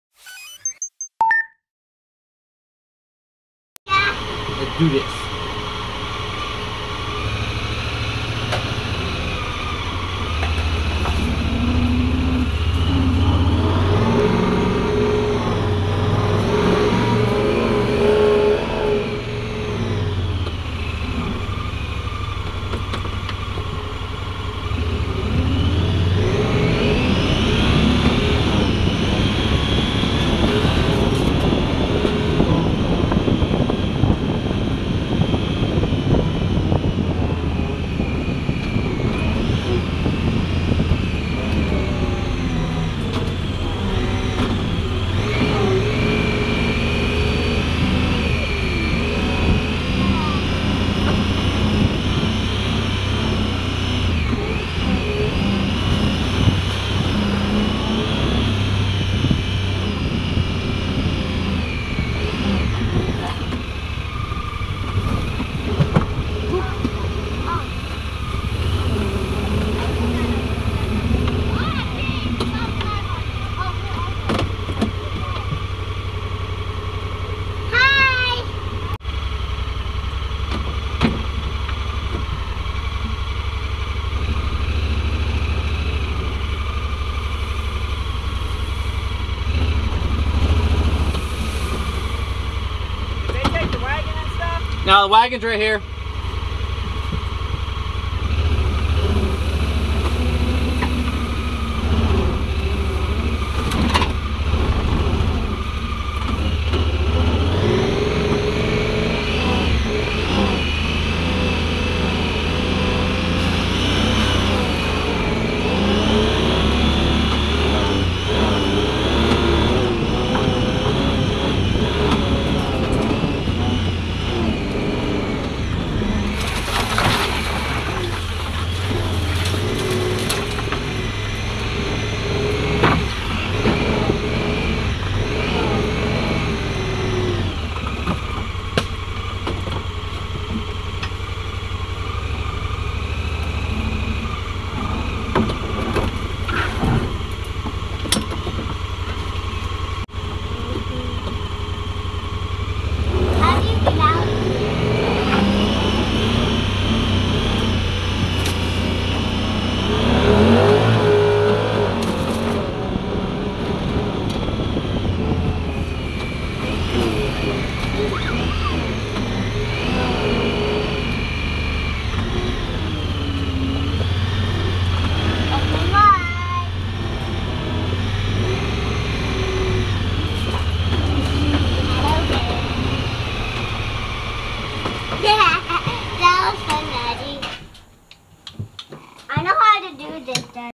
Pioneer 1000-5 Garmin VIRB test
Trying out the VIRB in my Honda Pioneer 1000-5.

Mounted inverted on the ROPS